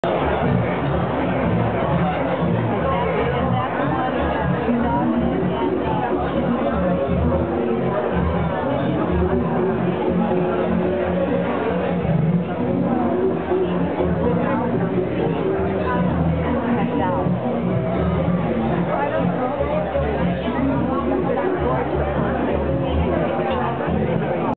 Cafe des los Angelitos
Late night dinner at the Cafe des los Angelitos.